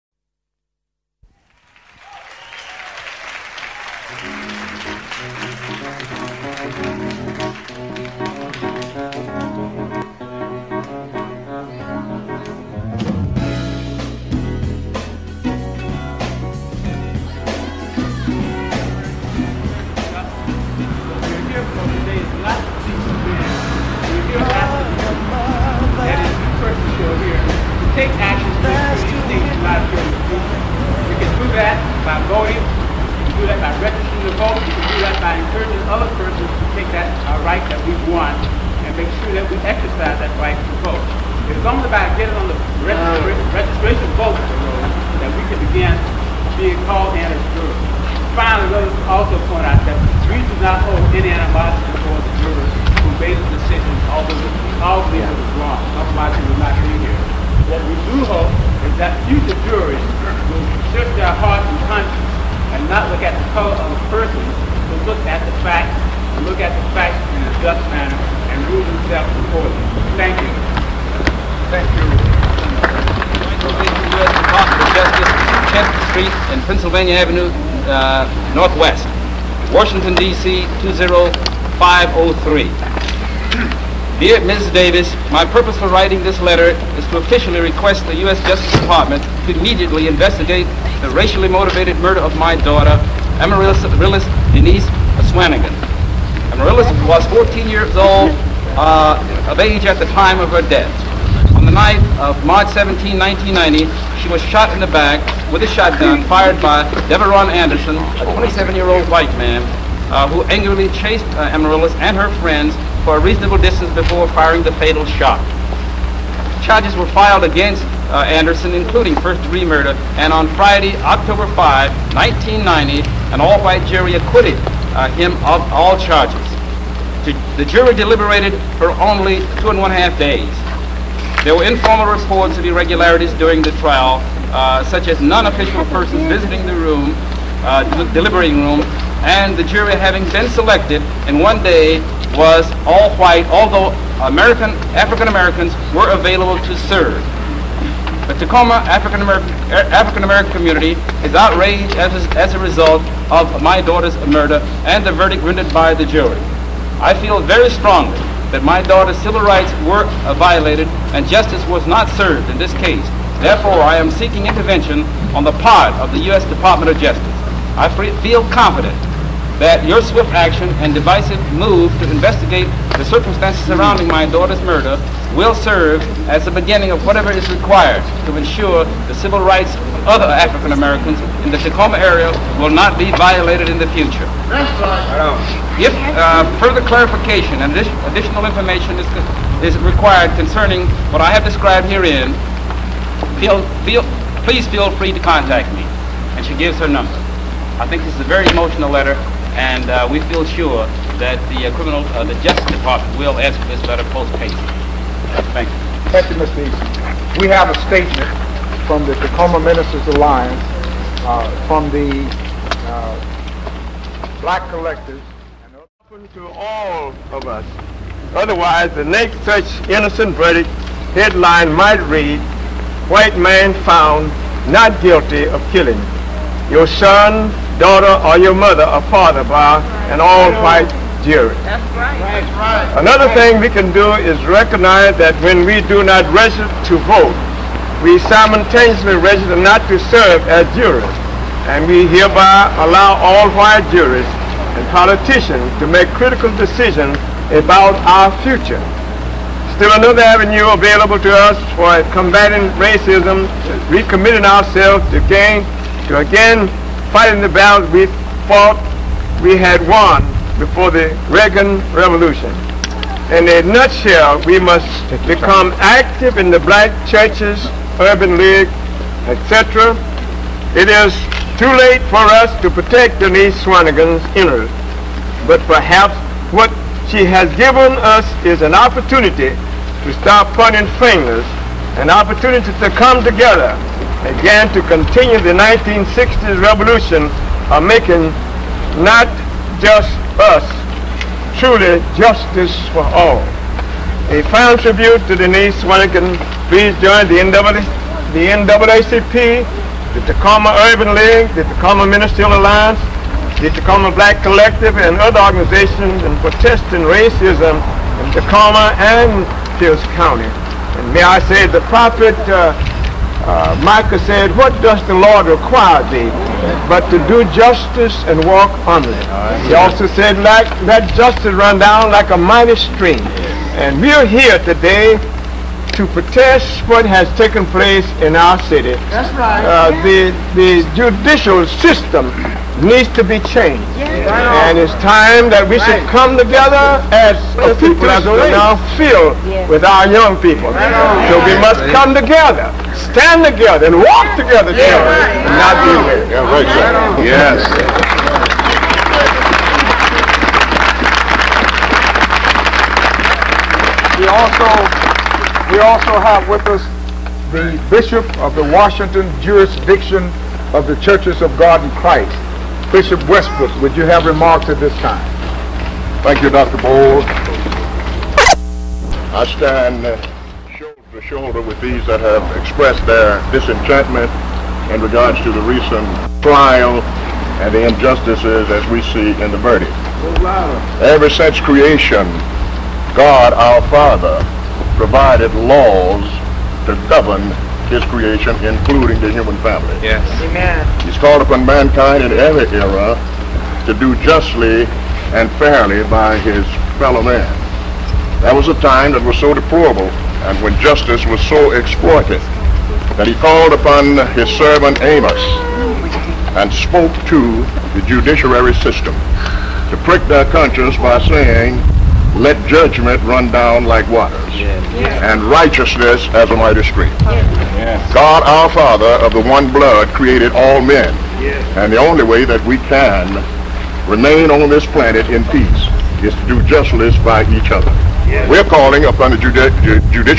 Denise Swannigan part 1
This is a march for Denise Swannigan. A 14 year old African-American female who was shot in the back by Devaron Anderson a 30 year old white male. He was acquitted of all charges.